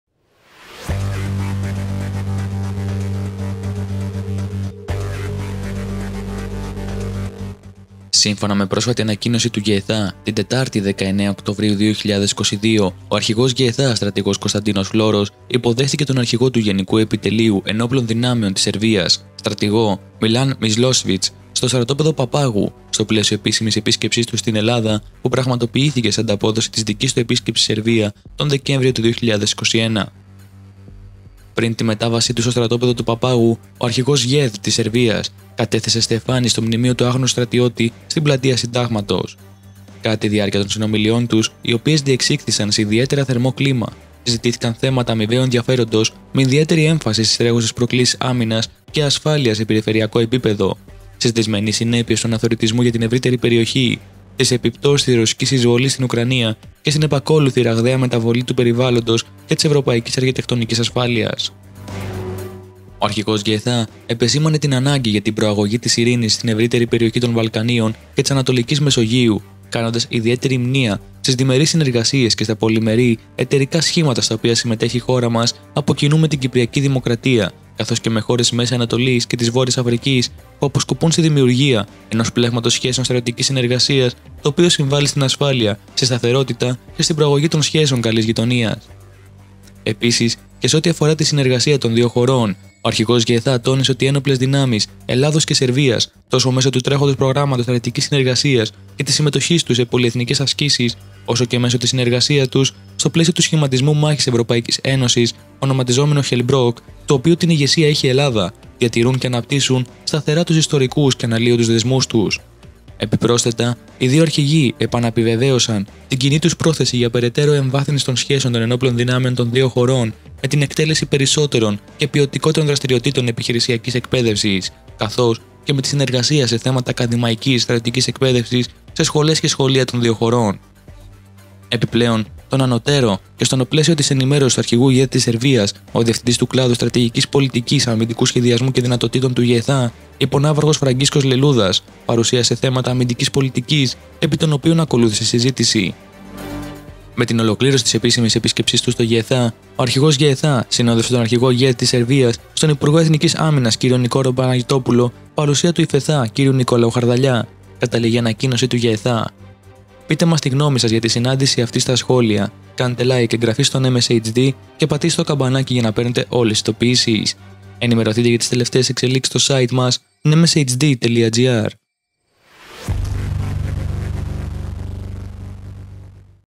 ΘΕΤΙΚΟ ΒΗΜΑ: Ελλάδα και Σερβία ενισχύουν τους στρατιωτικούς δεσμούς τους
Σύμφωνα με πρόσφατη ανακοίνωση του ΓΕΕΘΑ «Την Τετάρτη 19 Οκτωβρίου 2022, ο Αρχηγός ΓΕΕΘΑ Στρατηγός Κωνσταντίνος Φλώρος υποδέχθηκε τον Αρχηγό του Γενικού Επιτελείου Ενόπλων Δυνάμεων (ΓΕΕΔ) της Σερβίας Στρατηγό Milan Mojsilović στο Στρατόπεδο «ΠΑΠΑΓΟΥ», στο πλαίσιο επίσημης επίσκεψής του στην Ελλάδα, που πραγματοποιήθηκε σε ανταπόδοση της δικής του επίσκεψης στην Σερβία τον Δεκέμβριο 2021
📰Διαβάστε την είδηση | NEMESIS HD

00:00 INTRO
00:10 ΘΕΤΙΚΟ ΒΗΜΑ: Ελλάδα και Σερβία ενισχύουν τους στρατιωτικούς δεσμούς τους
02:35 OUTRO

PHOTOS / VIDEOS:
ΓΕΕΘΑ